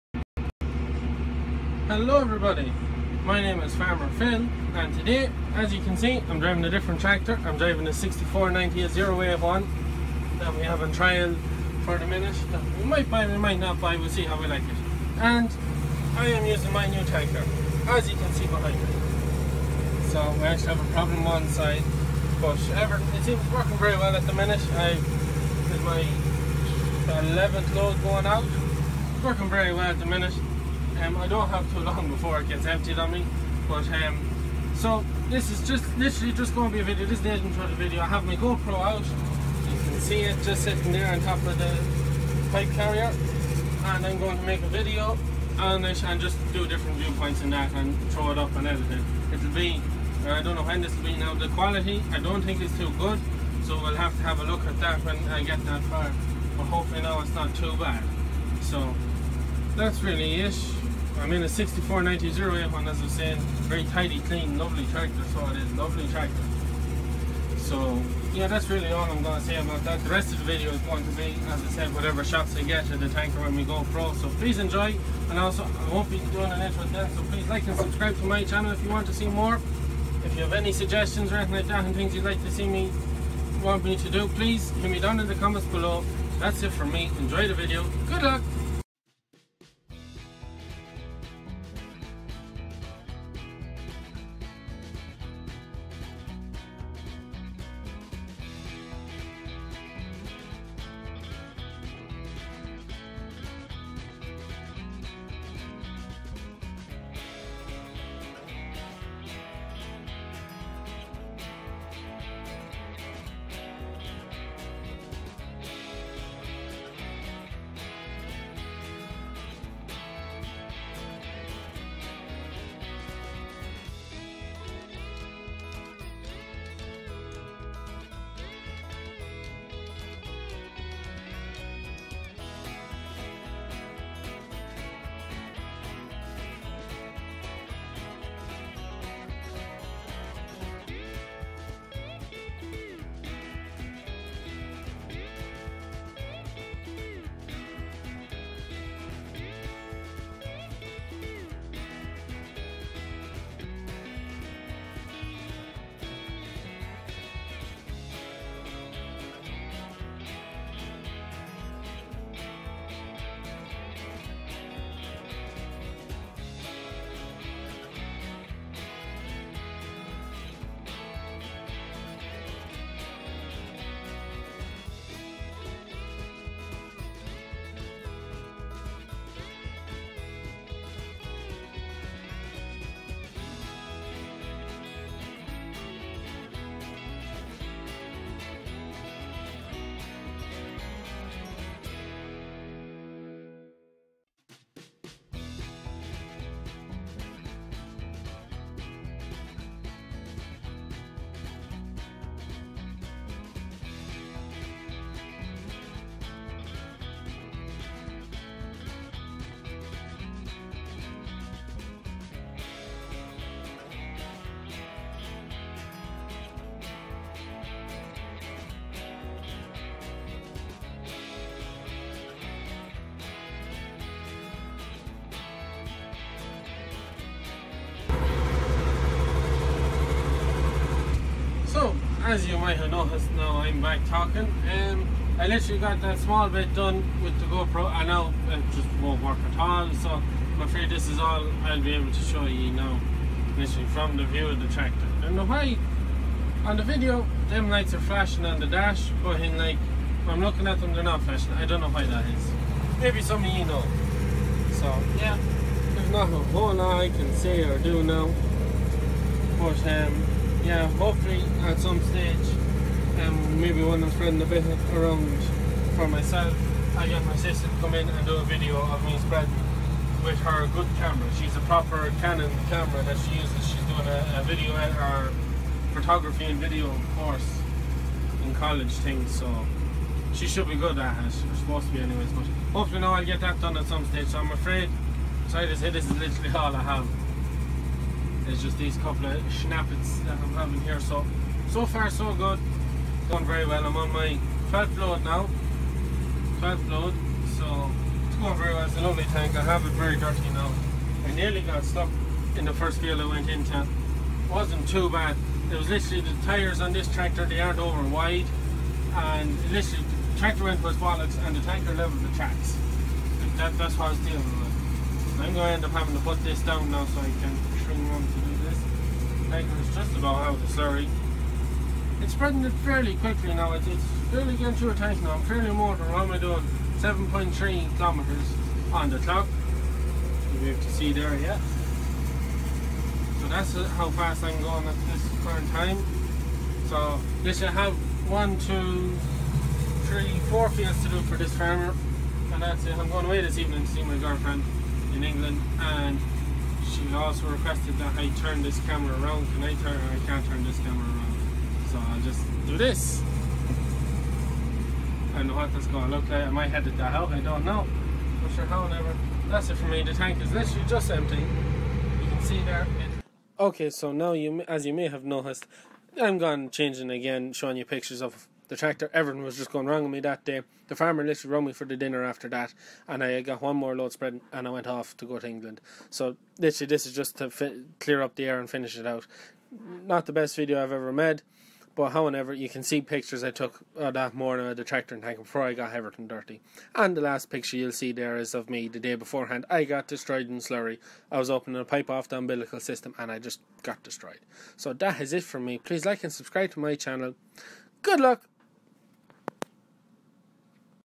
Using my new dribble bar tanker
this is a video of my new abbey 2500r premium plus dribble bar tanker in use for the first time. sorry about video quality everything seemed to be going again me that day.
and also on snapchat: farmerphil135
and on twitter at FARMERPHIL135